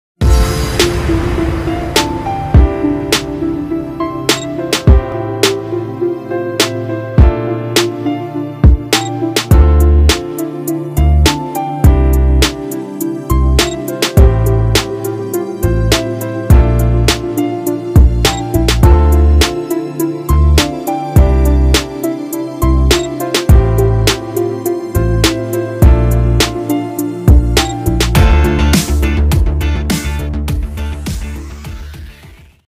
участок гибридизации подсолнечника 2020
уч гибридизации гранстаростойкого подсолнечника в 2020 году до пояса не достает